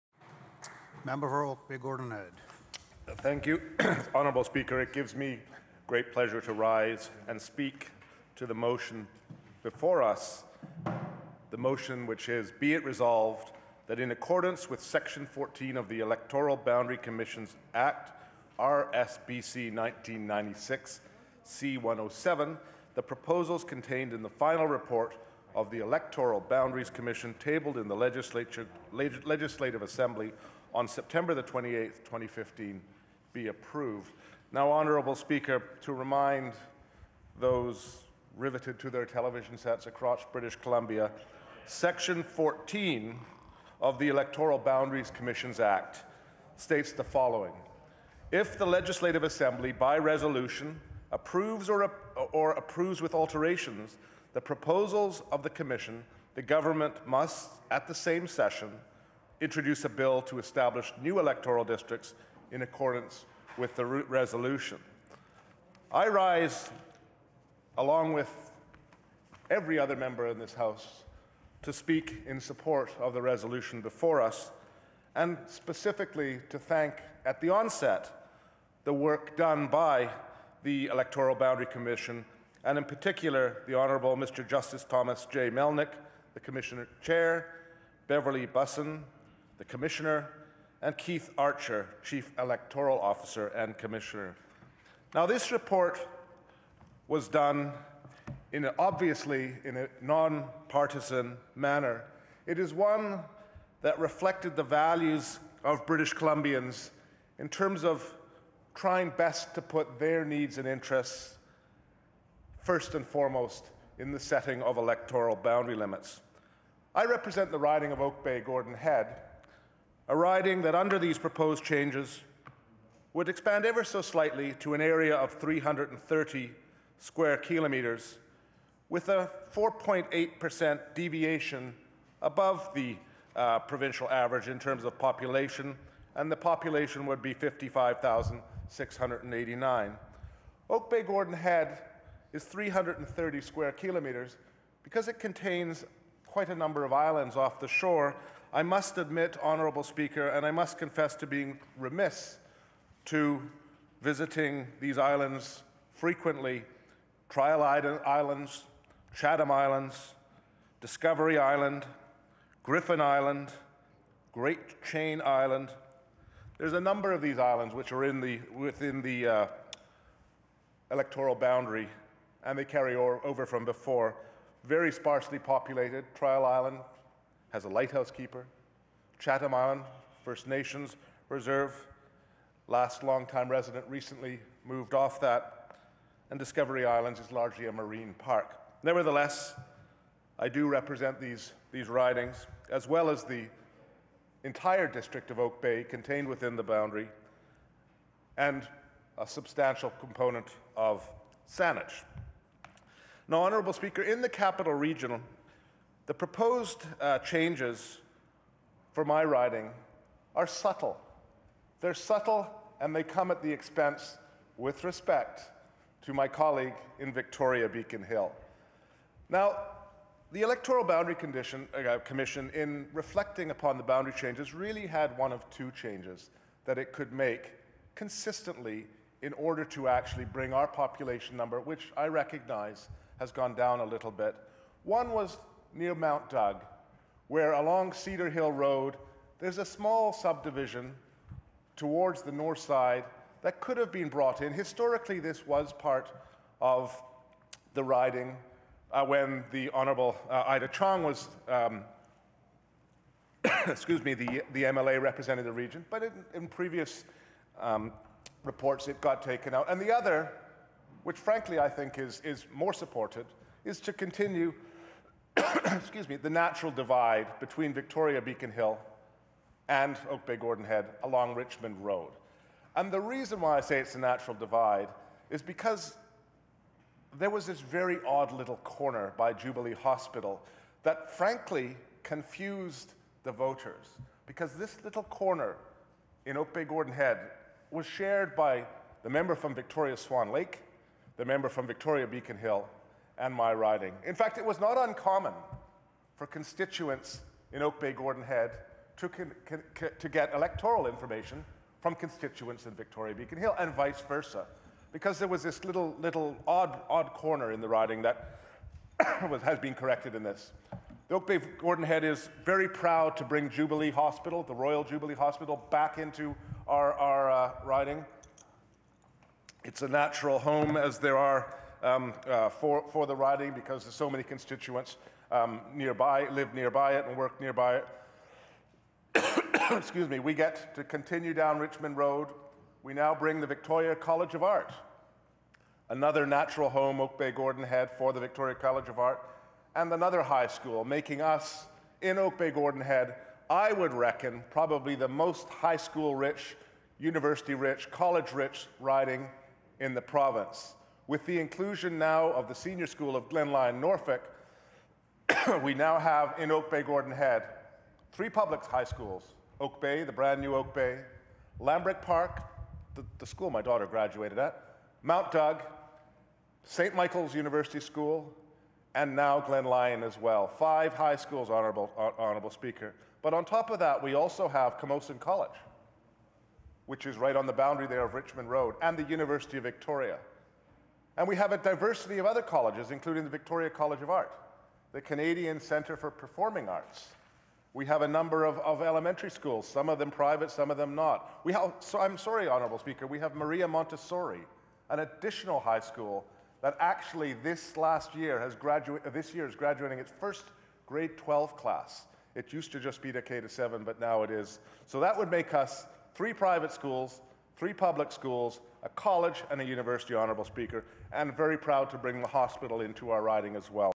Debating Motion to Support Electoral Boundary Commission Report
A snippet of the debate to support Electoral Boundary Commission Report